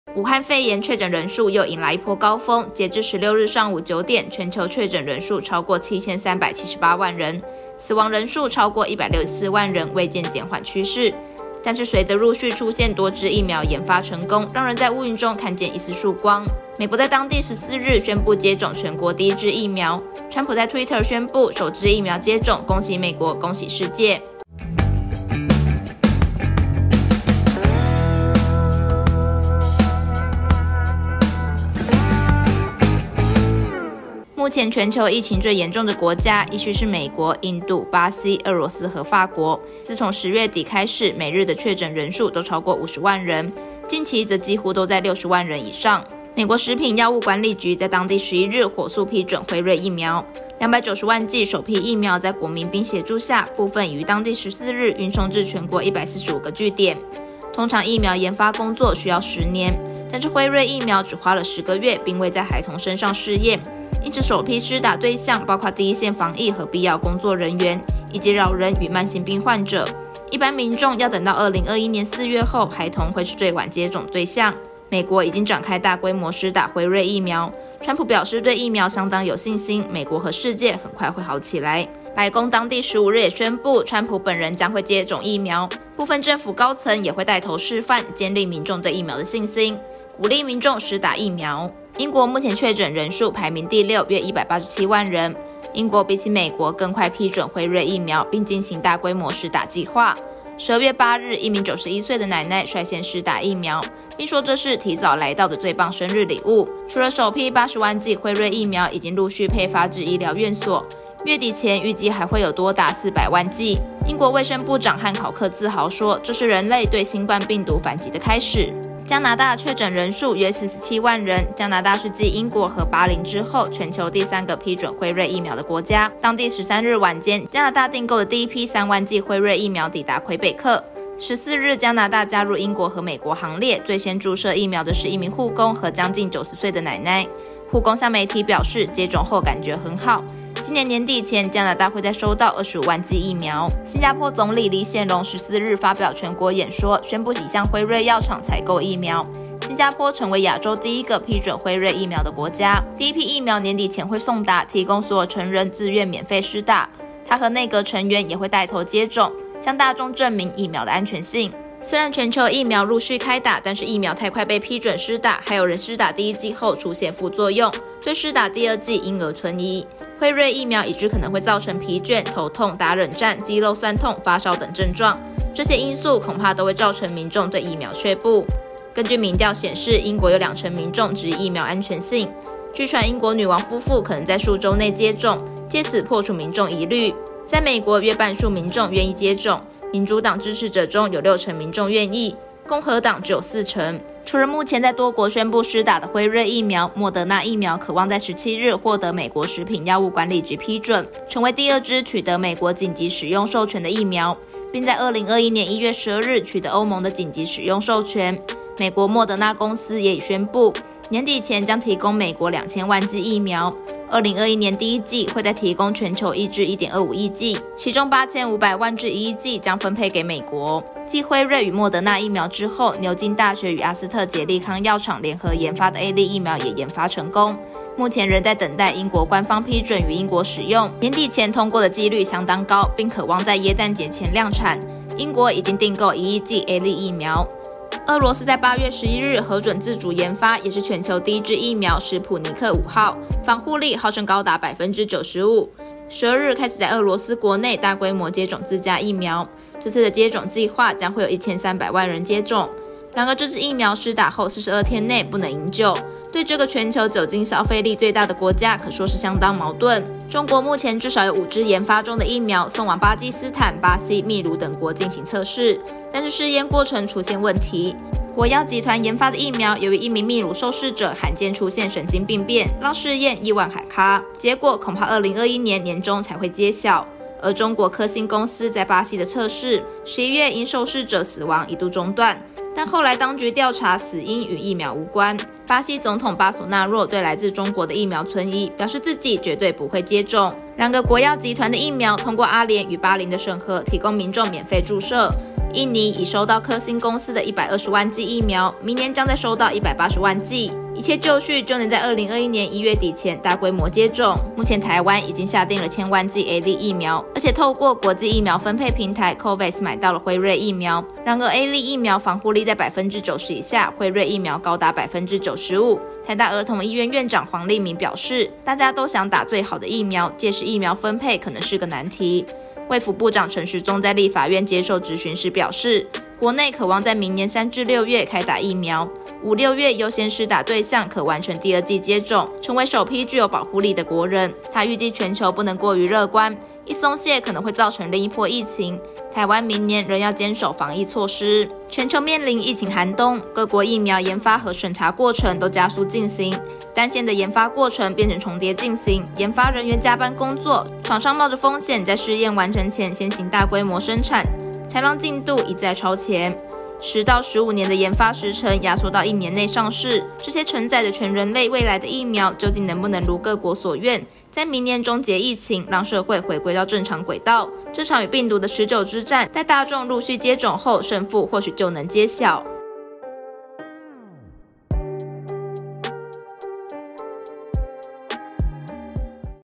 《即時說新聞》疫苗來了！ 各國大規模接種計畫陸續開跑
武漢肺炎（新型冠狀病毒病，COVID-19）確診人數又迎來一波高峰，截至15日上午10點，全球確診人數超過7318萬人，死亡人數超過162萬人，未見減緩趨勢，但是隨著疫苗研發陸續成功，讓人在烏雲中看見一絲曙光。